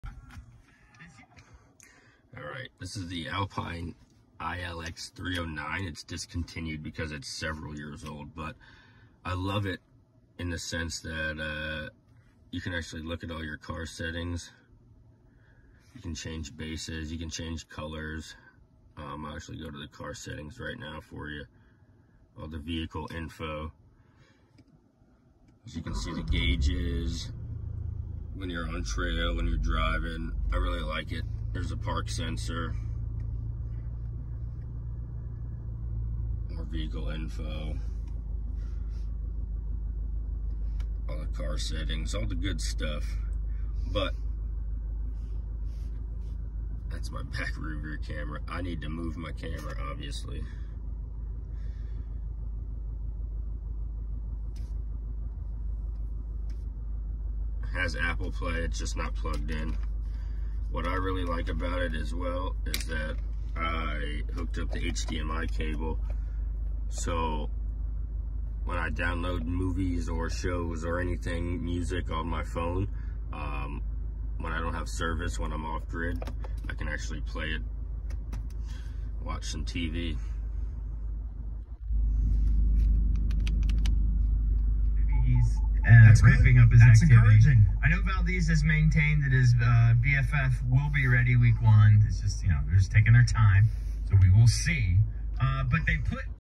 Upgraded iLX-f309 Alpine head unit. 2022 Subaru Forester Wilderness
iLX-f309 ￼ is it discontinued, I’ve had this radio for about four years now, they moved on to the newer models, the 409. However, this one works beautifully.I love it and I highly recommend upgrading..

I had one in my 2017 Crosstrek, when I sold it, I sold it with everything.
So when I bought my 2021 Forrester sport, immediately re-bought and went and got it installed. When I sold that I got it removed so I could reinstall it in my 3rd Subie, the wilderness.

Subaru really dropped the ball with the infotainment system when it comes to the Crosstrek and Forester.